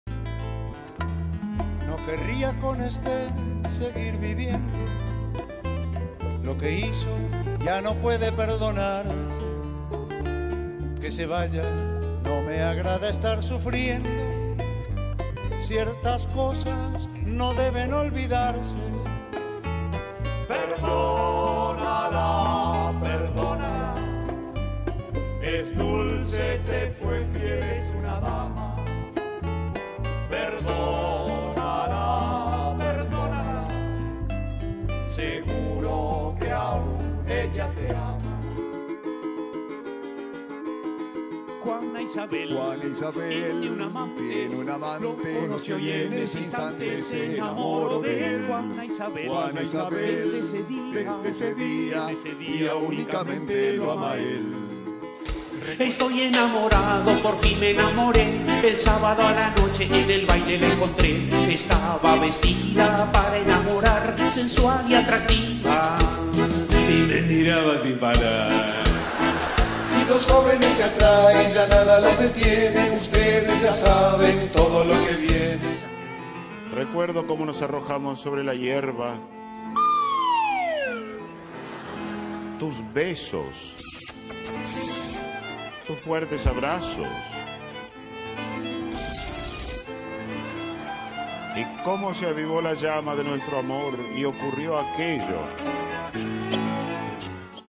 Les Luthiers, Premio Princesa de Asturias 2017 | Cultura
El premio Princesa de Asturias de Comunicación y Humanidades ha recaído este miércoles en Les Luthiers.